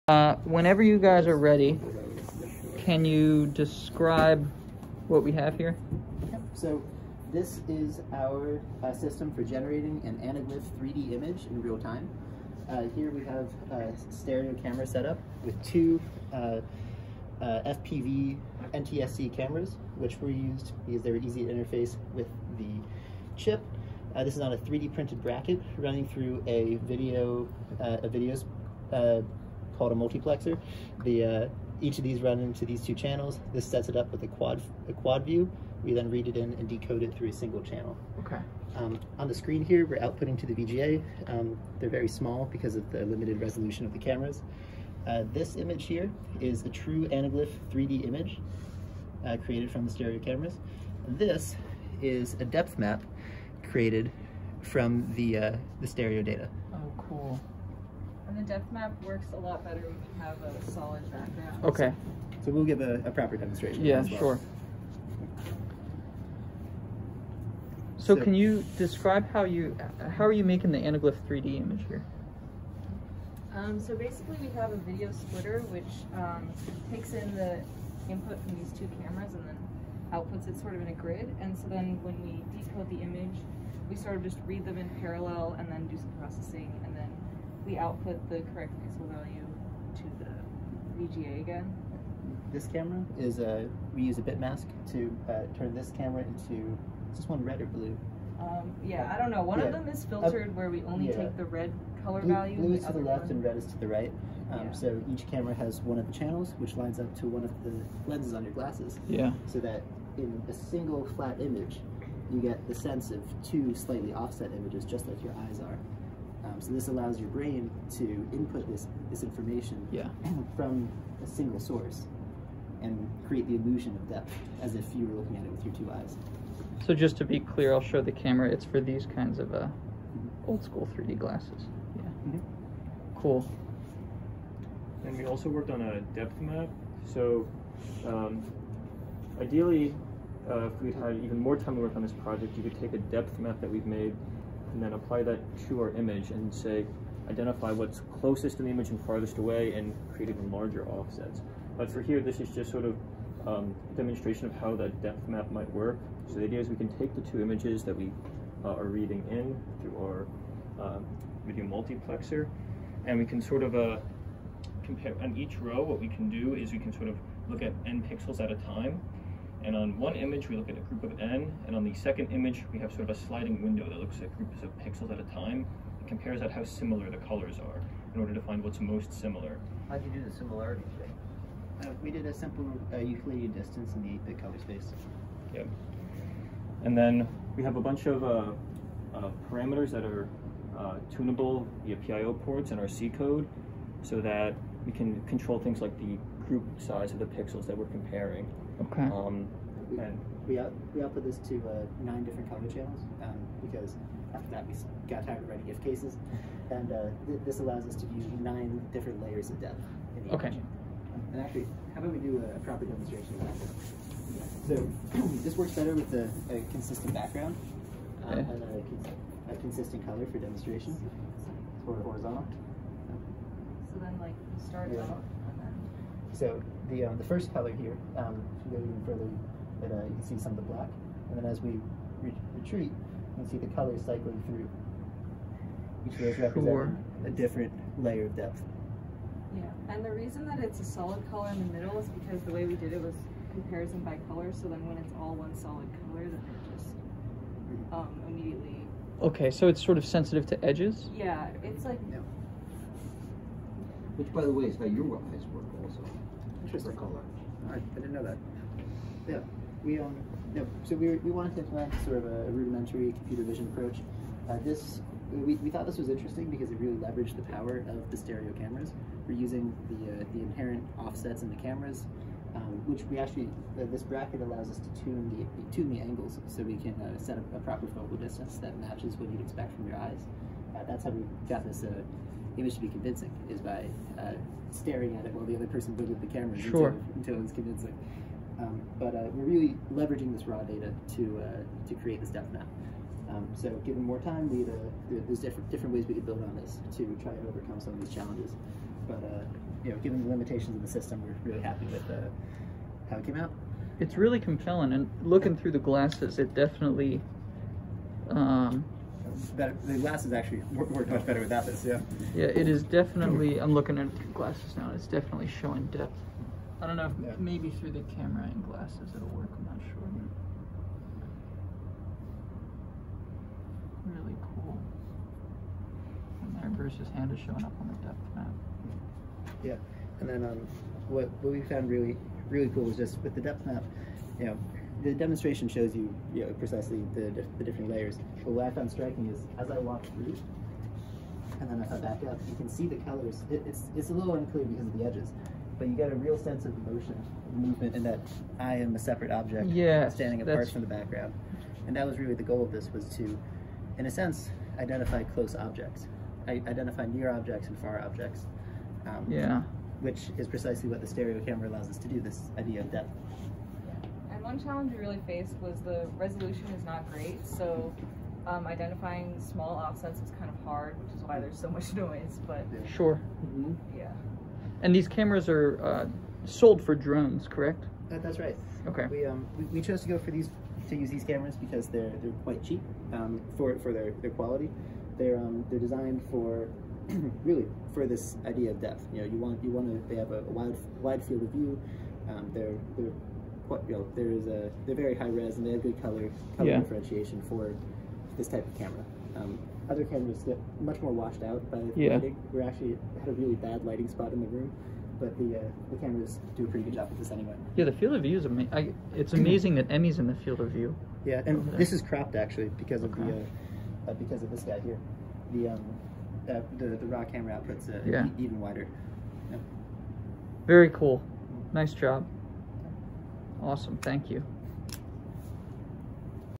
Realtime anaglyph 3D video and depth mapping using the DE1-SoC
Final project from ECE 5760, Spring 2022

Jack Defay, Emmi Wyttenbach, and Eric Kahn demonstrate a system for generating realtime anaglyph 3D video and depth maps using the DE1-SoC.